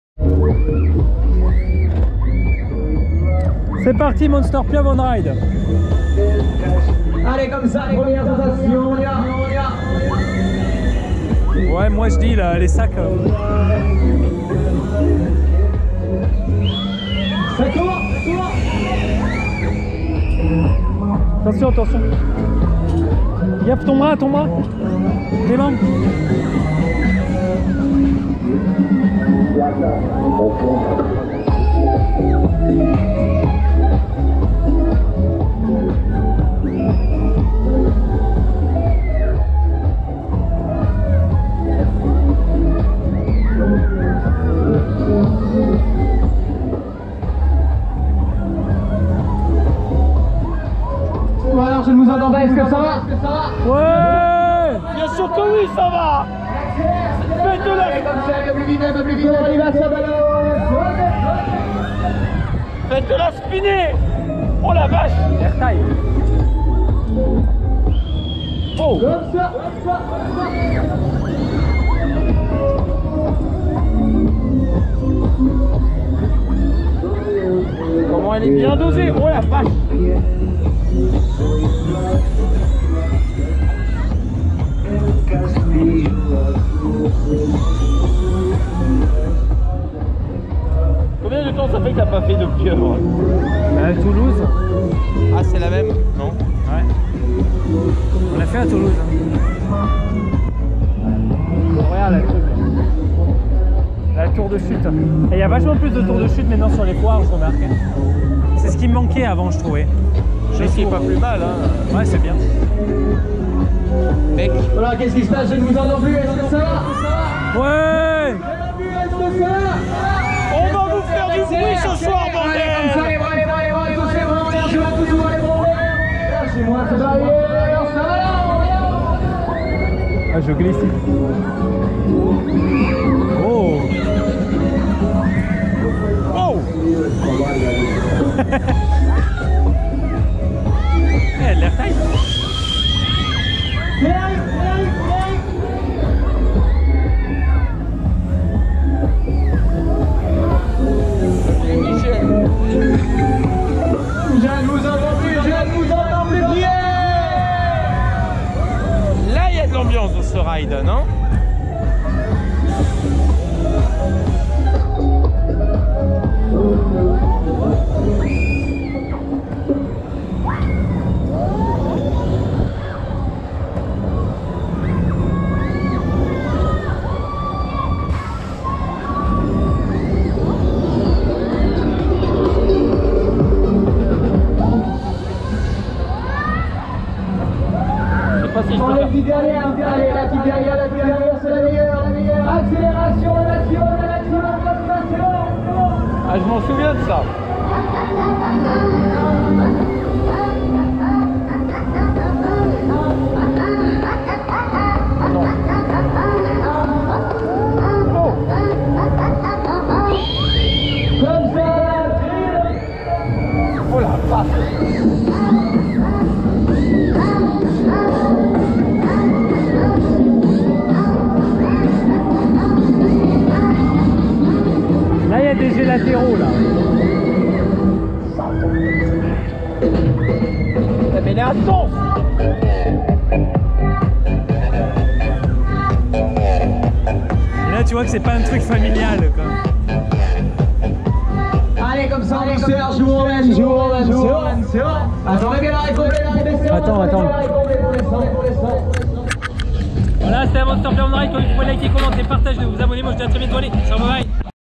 MONSTER PIEUVRE [Onride] Perpignan 2019
Video onride de la Monster Pieuvre à la fête foraine de Perpignan 2019